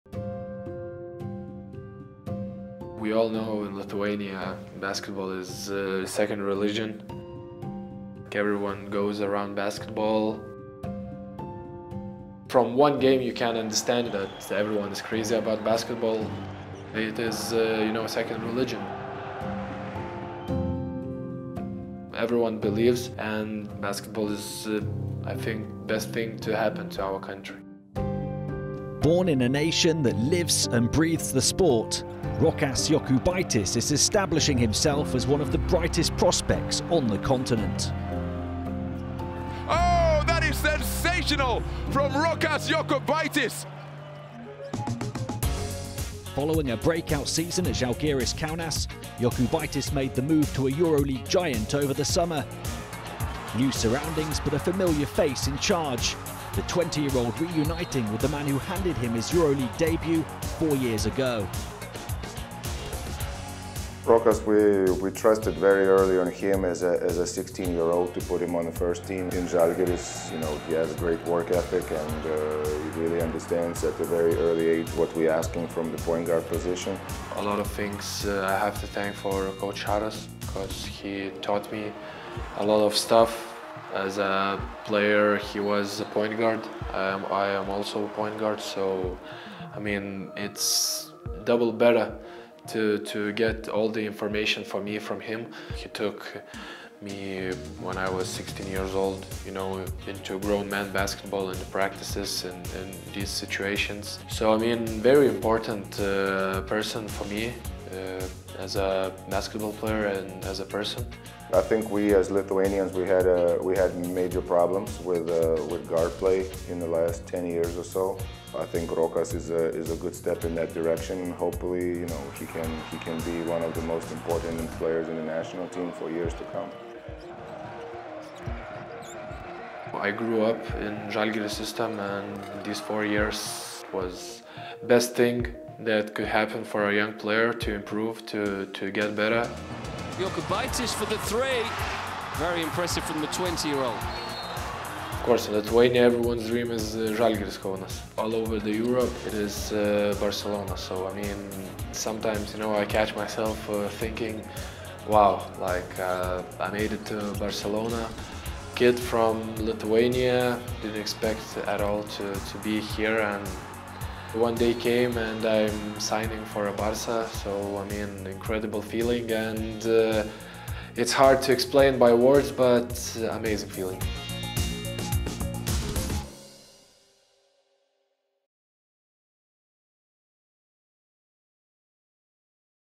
Focus on: Rokas Jokubaitis, FC Barcelona
FC Barcelona guard Rokas Jokubaitis is delighted to have joined FC Barcelona and playing for Coach Jasikevicius, the man who took him under his wing when he was 16 years old.